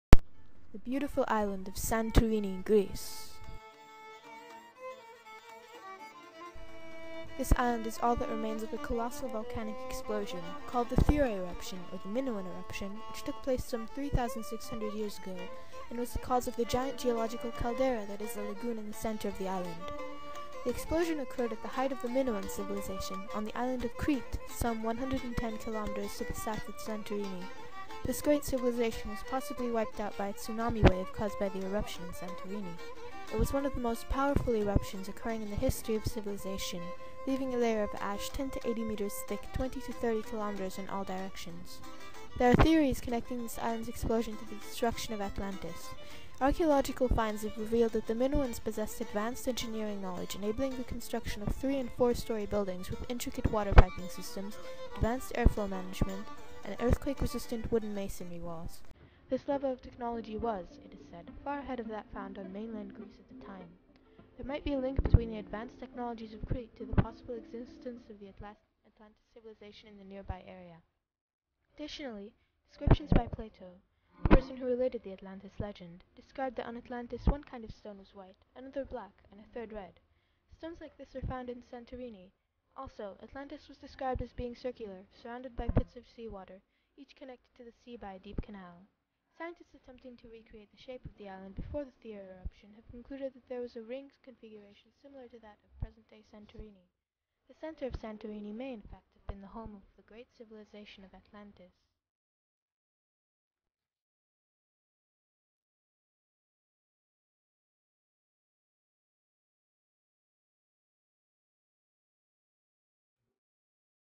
Santorini movie 0001
The beautiful Island of Santorini, Greece
This island is all that remains of a colossal volcanic explosion, called the Thera eruption, or the Minoan eruption, which took place some 3,600 years ago, and was the cause of the giant geological caldera that is the lagoon in the center of the island.
The explosion occurred at the height of the Minoan civilization, on the island of Crete, some 110 km to the south of Santorini.  This great civilization was possibly wiped out by a tsunami wave caused by the eruption in Santorini. It was one of the most powerful eruptions occurring in the history of civilization, leaving a layer of ash 10-80 m thick 20-30 km in all directions.
There are theories connecting this island's explosion to the destruction of Atlantis.  Archaeological finds have revealed that the Minoans possessed advanced engineering knowledge enabling the construction of three- and four-story buildings with intricate water piping systems, advanced air-flow management, and earthquake-resistant wood and masonry walls. This level of technology was, it is said, far ahead of that found on mainland Greece at the time. There might be a link between the advanced technologies of Crete to the possible existence of the Atlantis civilization in the nearby area.
Additionally, descriptions by Plato (the person who related the Atlantis legend) described that on Atlantis "one kind of stone was white, another black, and a third red"-stones like this are found in Santorini.  Also, Atlantis was described as being circular, surrounded by pits of sea water, each connected to the sea by a deep canal.  Scientists attempting to recreate the shape of the island before the Thera eruption have concluded that there was a ring configuration similar to that of Present day Santorini.  The center of Santorini may in fact have been the home of the great civilization of Atlantis.